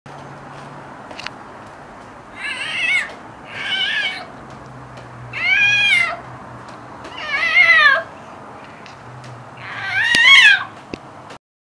Jessica The Cat
Jessica showing off her cat outfit.